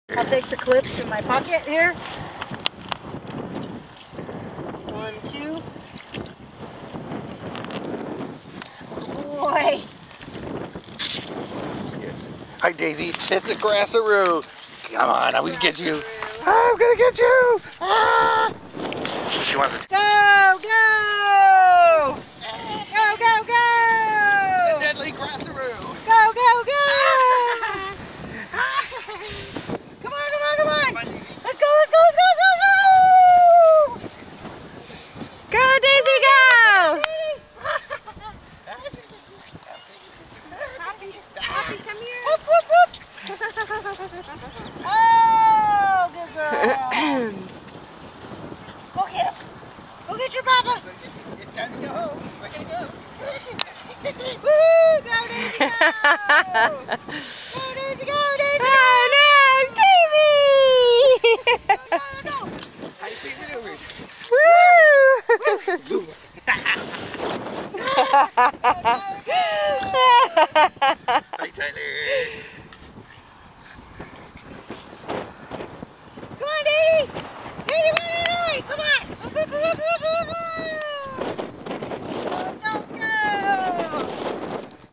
Point Isabelle Grass-a-Roo
Princess Daisy and Tyler at the local shoreline park for a walk, some off road training.
After training we stopped at the grassy field so that Crazy Daisy can come out and do her Grass-a-Roo routine

Princess Daisy and her dark knight Sir Tyler are Pug PROS rescue dogs

Training by Chris and Lisa of Camp Harmony Dogs